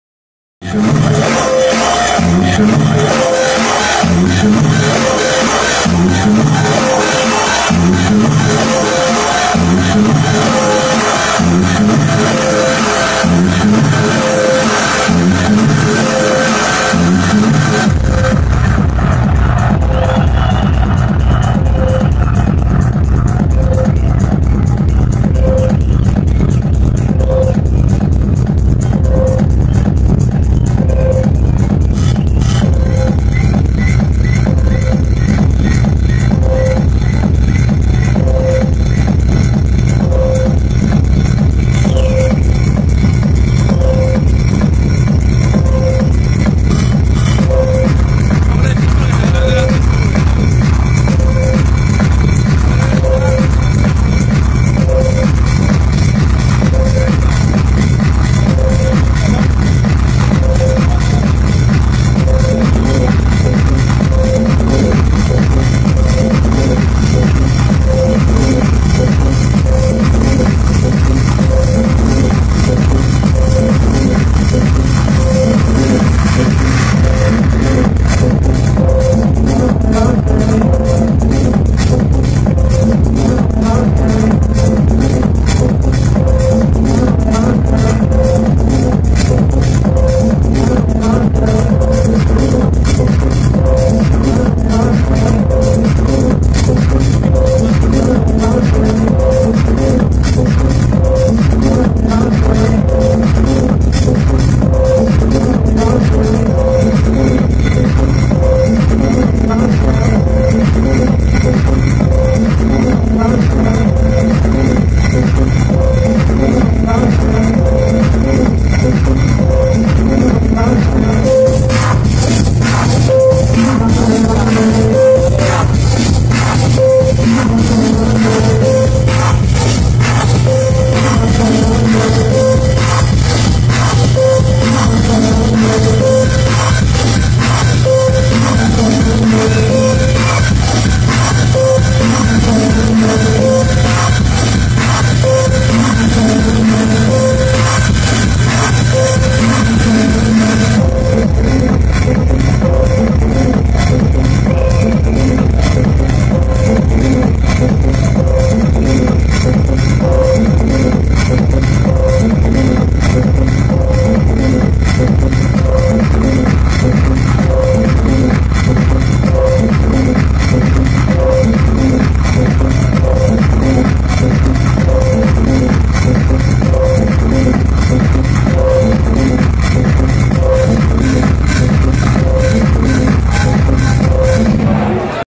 Len Faki @ HEX Barcelona (29-07-2016)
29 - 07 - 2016
HEX presents: LEN FAKI w/ Pøli, Recanism, Hitch

HEX at Input, in the architectural museum of Barcelona: Poble Espanyol.

by Nil Calvarons Photography

visuals: Acid Thermal